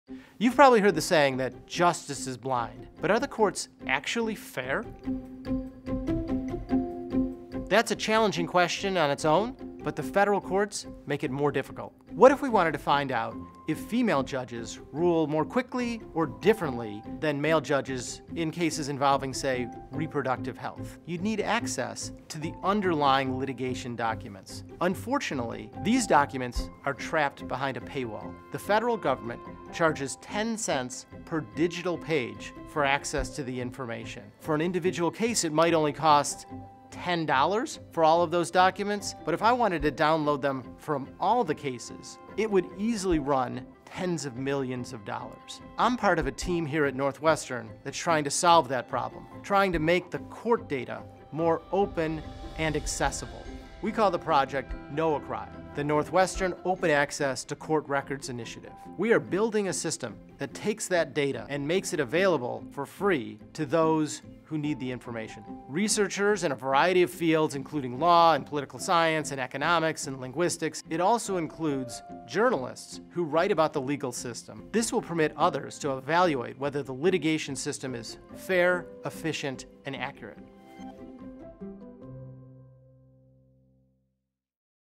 A Northwestern Law Professor Explains: Access to Court Records
Professor David Schwartz explains how federal court system paywalls derail researchers who want to study patterns in court records. The cross-disciplinary Northwestern Open Access to Court Records Initiative (NOACRI) is hoping to change all that.